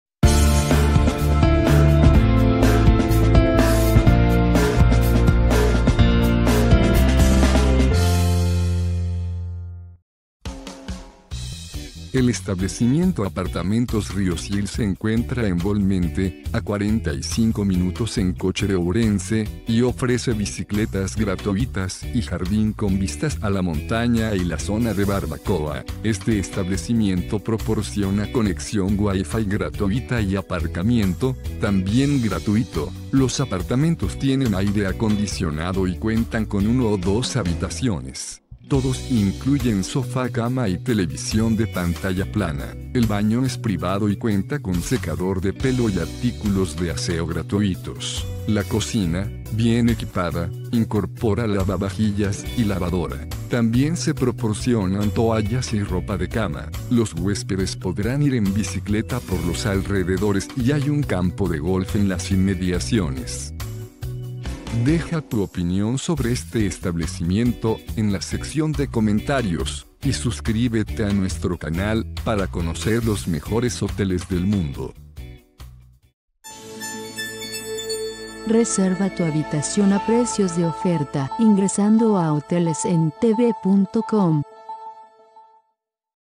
Apartamentos Rio Sil en Bolmente
Apartamentos Rio Sil ofrece 3 habitaciones con caja fuerte, aire acondicionado y secador de pelo. La edificación de Apartamentos Rio Sil se encuentra localizada en una encantadora región de Bolmente.